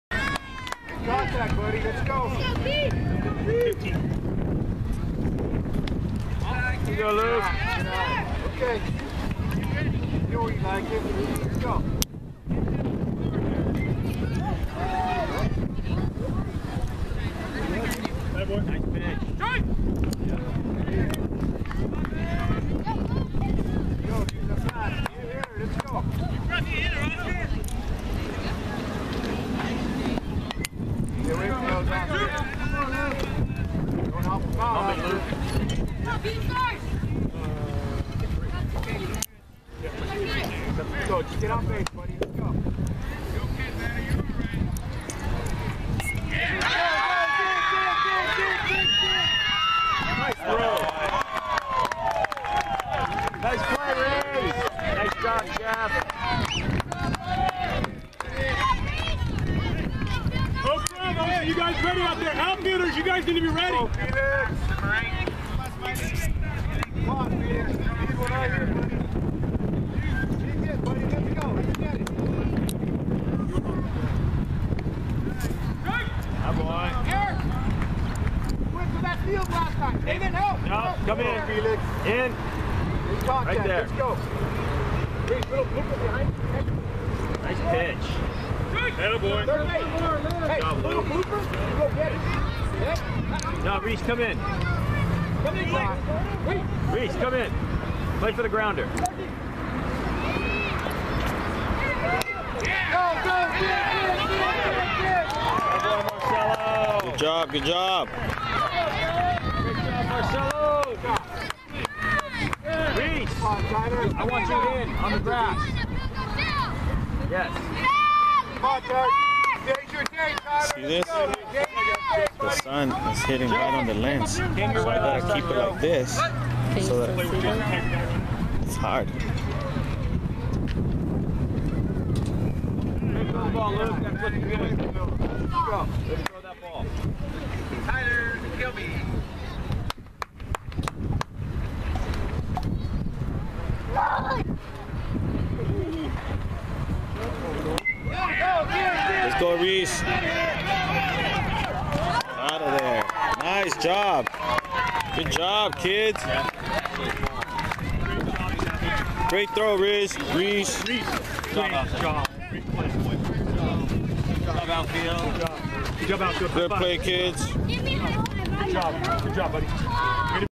Oakgrove All Stars vs. Spartan All Stars - 4th Bottom
Video# HDV 0028 - District 59 - San Jose, CA
9-Year Old All Stars - Video taken 06/17/13
Oakgrove wins 8-4